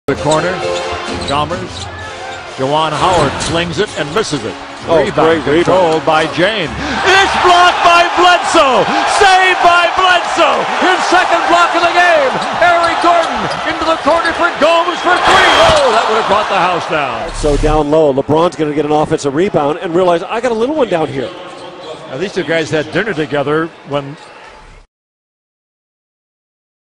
Eric Bledsoe HUGE block on LeBron James (Jan. 12, 2011)
6'1" Eric Bledsoe goes straight up for the enormous rejection on LeBron James.  Oh, and the Clippers won. There's some karma for you LeBum..haha Only the sickest plays in basketball.